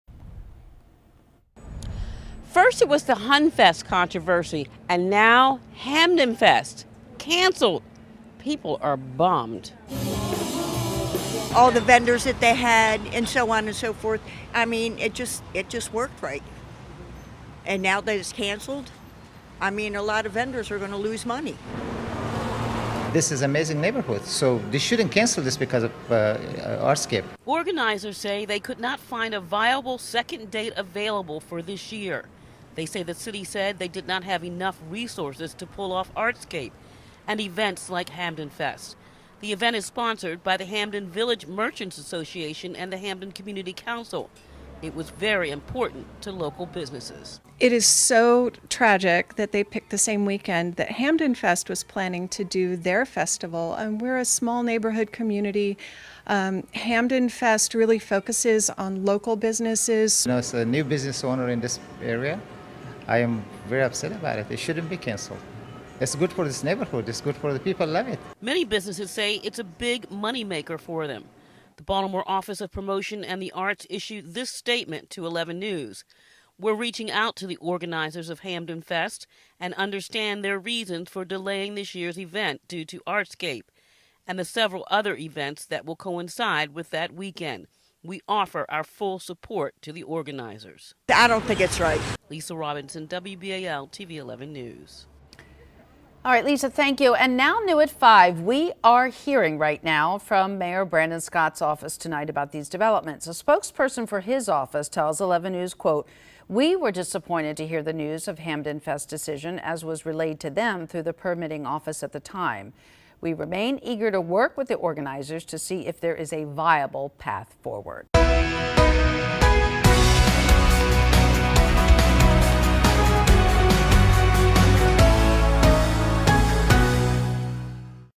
Hampdenfest canceled for 2023 due to Artscape conflict
Hampdenfest will not happen this year. Organizers said their application was denied thanks to Artscape.  Organizers said Baltimore City asked them to change the date of the annual event because it was planned for the same weekend as Artscape. They chose to cancel.  The cancellation has left residents disappointed.